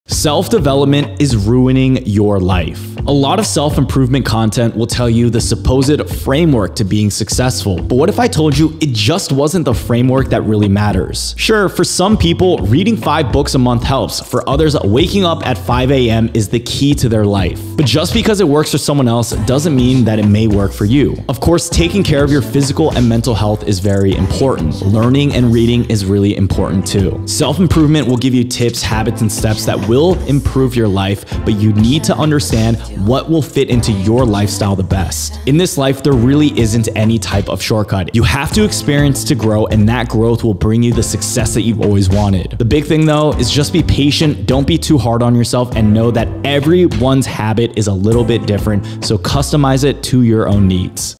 Self-Development Is Ruining Your Life
Have you ever thought about the impact of self-development content and books on your life? Well, in this video, I explain why they might actually be making your life harder.

Video Outline:
0:00 Intro
0:34 What Happened
6:56 Current Events
7:43 What About Crypto.com?
10:00 How to Protect Yourself
13:55 Sam Bankman-Fraud
16:48 My Thoughts
17:35 Outro